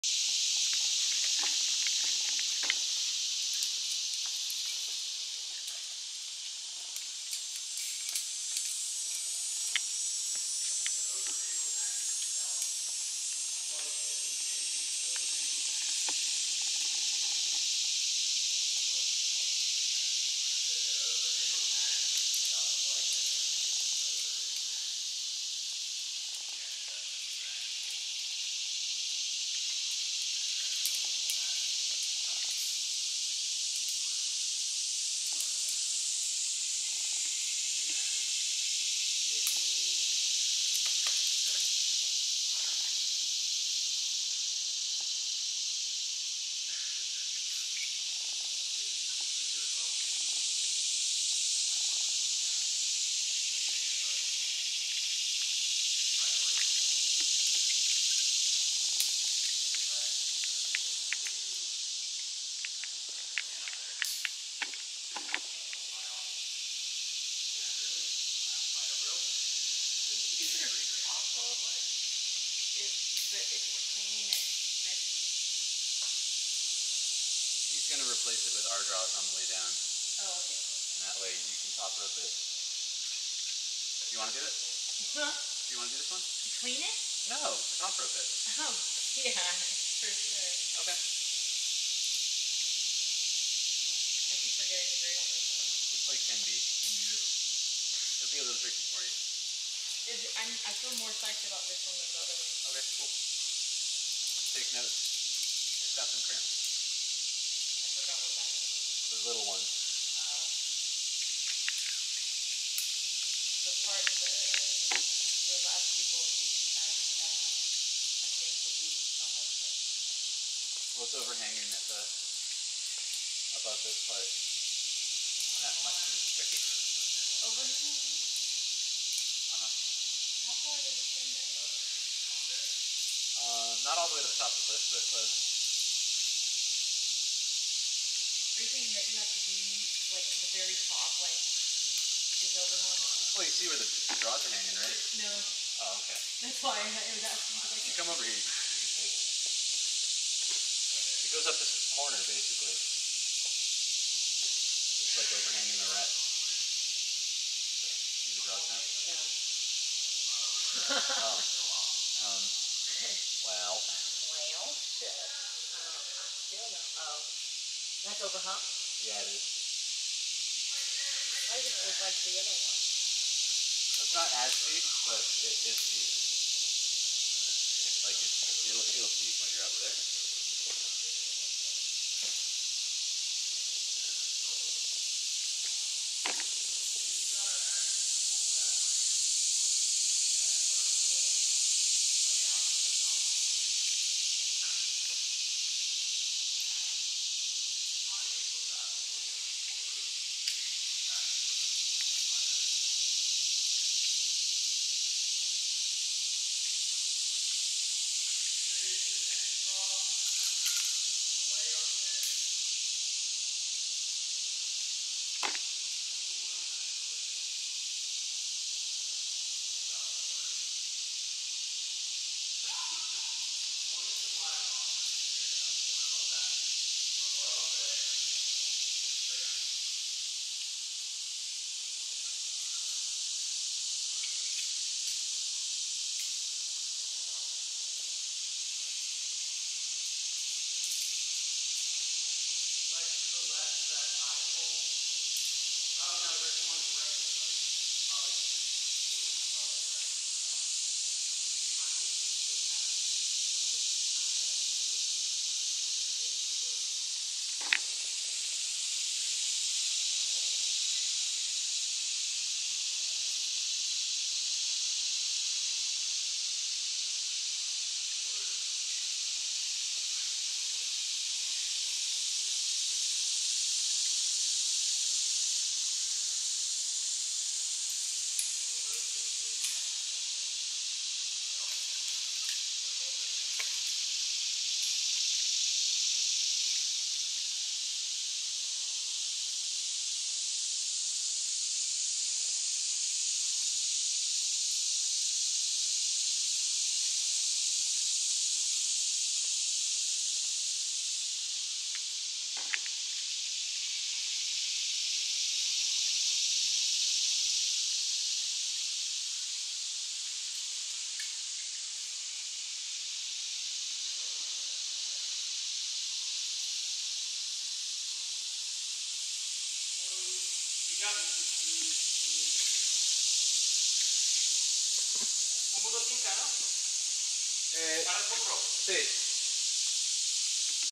5.10B / 6B A Brief history of Climb · The Gallery · Red River Gorge (KY) · USA
El mejor 6b hecho en mi vida!!! Increible la via!!!